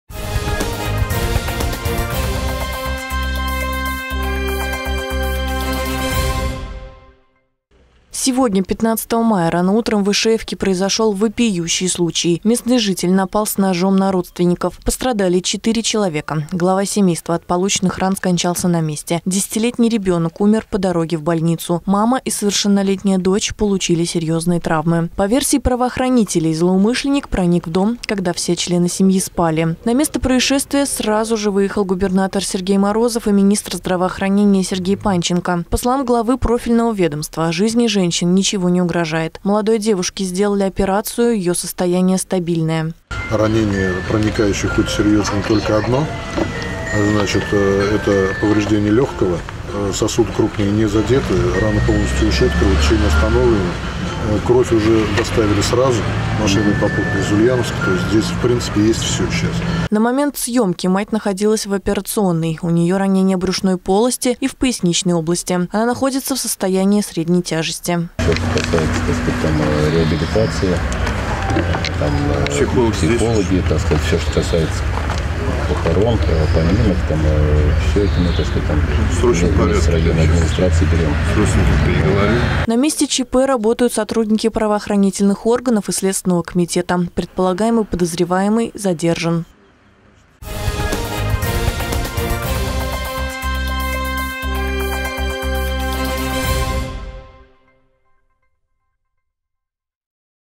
Сергей Морозов: «Всю помощь семье из Ишеевки берем на себя»
Сегодня, 15 мая, рано утром в Ишеевке произошел вопиющий случай. Местный житель напал с ножом на родственников. Пострадали четыре человека: глава семейства от полученных ран скончался на месте, 10-летний ребёнок умер по дороге в больницу, мама и совершеннолетняя дочь получили серьезные травмы.